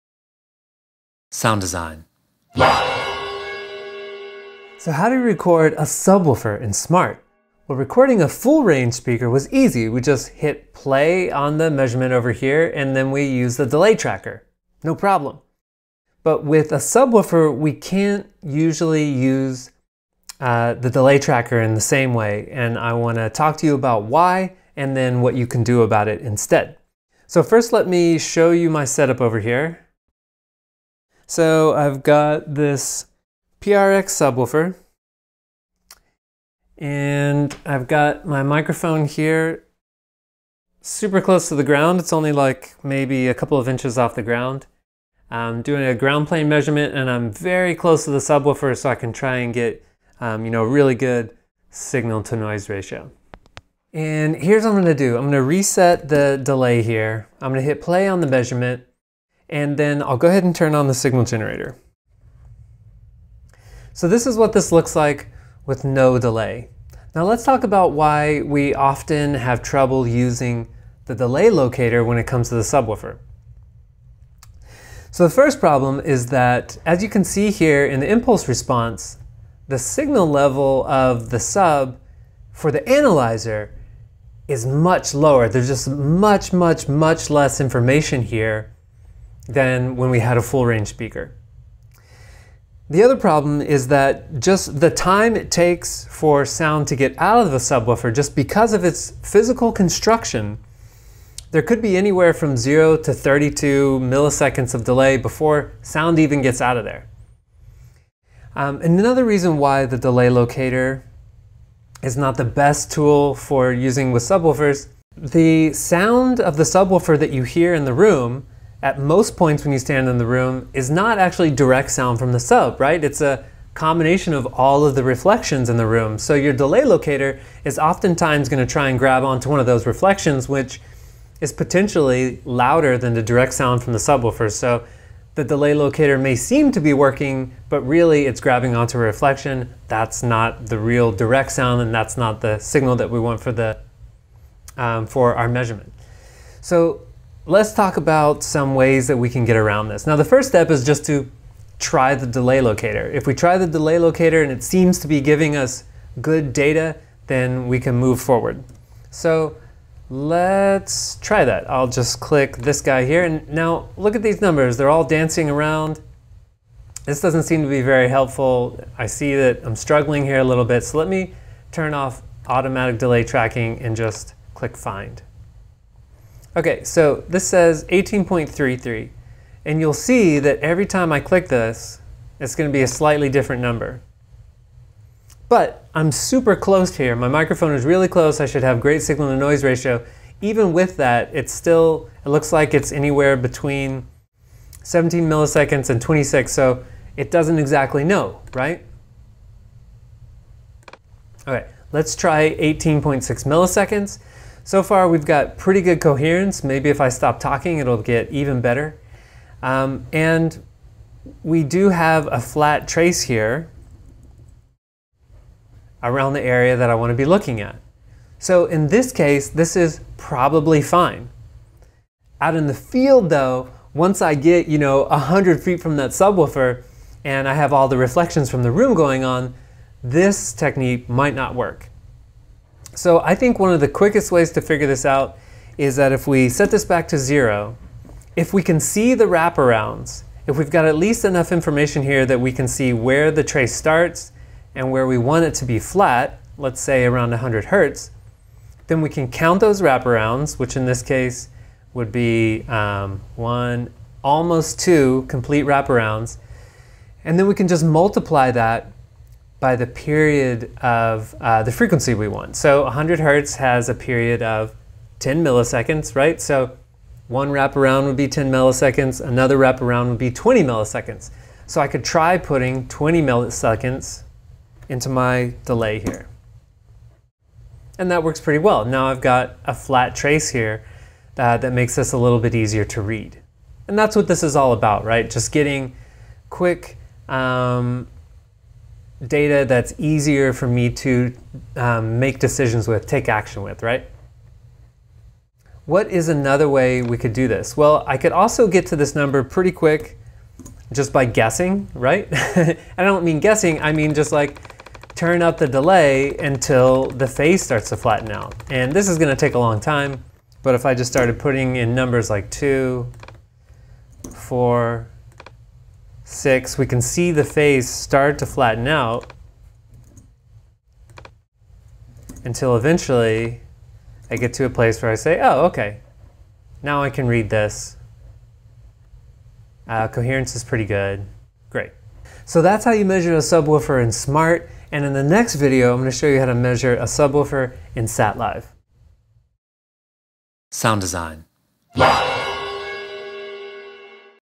How to set the delay locator to measure a subwoofer in Smaart [GSwSST29]
After you have practiced measuring a full-range speaker, the next step is to measure a subwoofer in Smaart. It turns out to be a little more tricky, but I've got some ideas to help you.